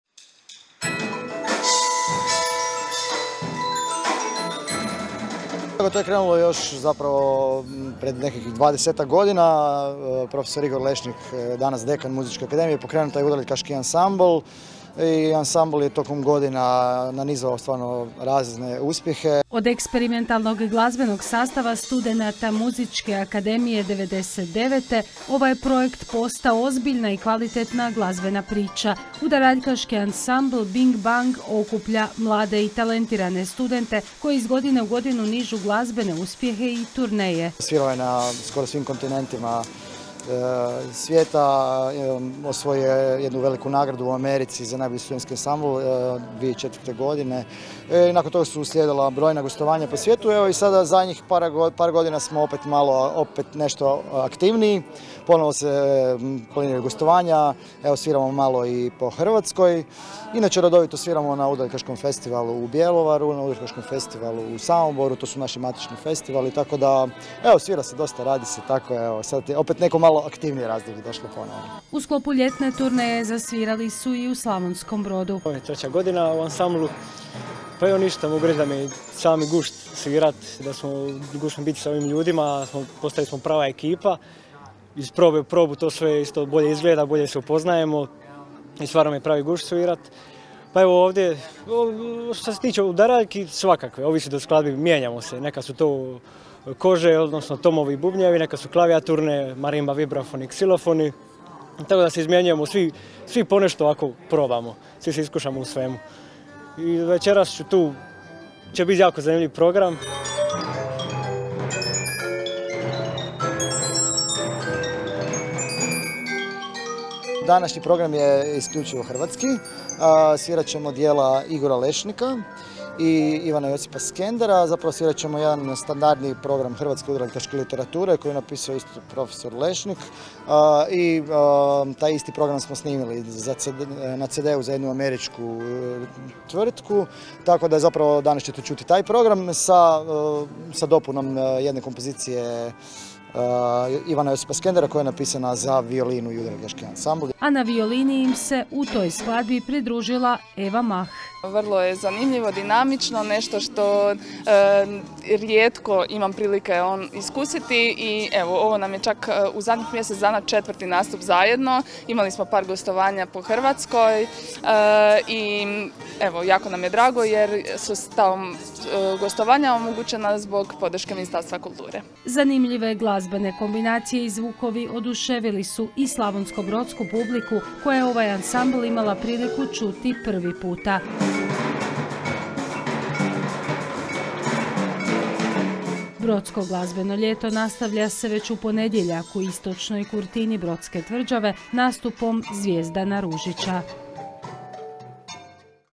SBTV - DNEVNIK - KONCERT UDARALJKAŠKOG ANSAMBLA BING BANG - 05.07.2024.
Brodsko glazbeno ljeto lagano ulazi u svoju završnicu. Sinoć je na platou ispred KKD nastupao Udaraljkaški ansambl Bing Bang iz Zagreba.

DNEVNIK SBTV-a | PON-PET | 20:00 i 22:00